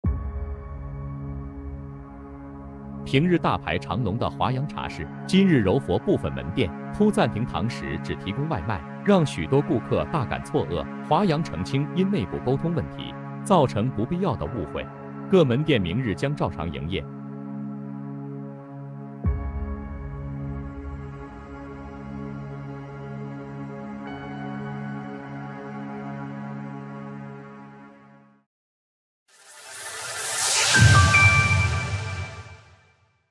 管理层及时回应息风波 柔佛华阳各门店将照常营业
平日大排长龙的华阳茶室，7月13日柔佛部分门店突暂停堂食，只提供外卖。华阳澄清因内部沟通问题，造成不必要的误会，各门店明日（7月14日）将照常营业。